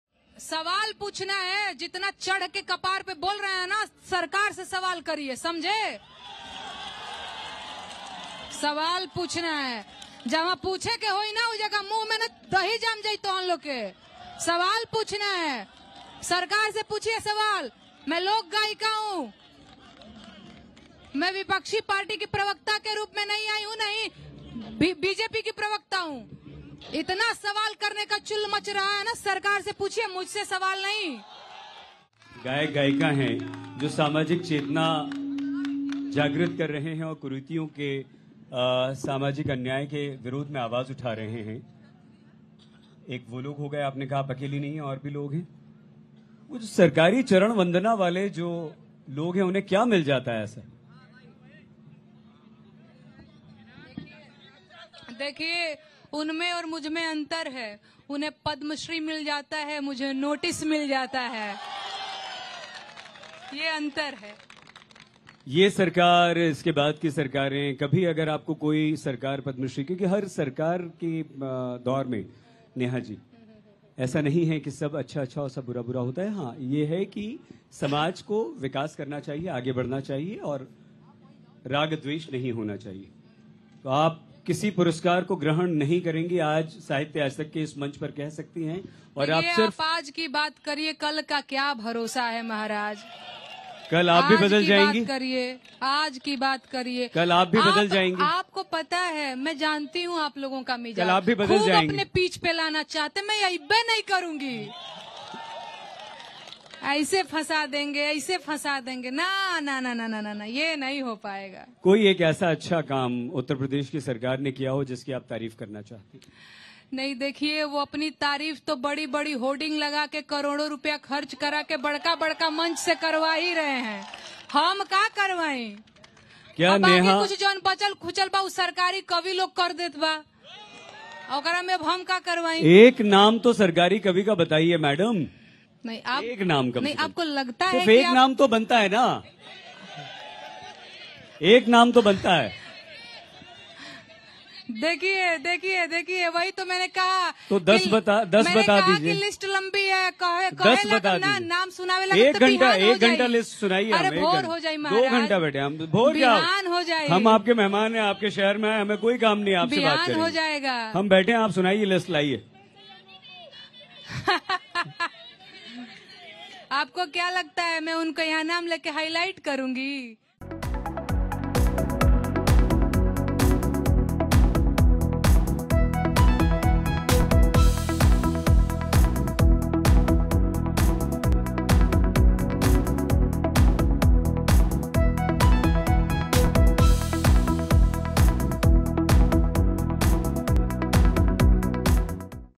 Neha Singh Rathor क्या नहीं लेगीं किसी सरकार से पुरस्कार? सुनिए नेहा का जवाब| Bihar Tak
नेहा सिंह राठौर जो इस वक्त चर्चाओं में हैं. साहित्य आज तक के कार्यक्रम में जब उनसे सवाल पूछा गया कि दूसरी सरकार आपको पुरस्कार देगी तो आप लेंगे या नहीं..देखें नेहा सिंह ने क्या जवाब दिया...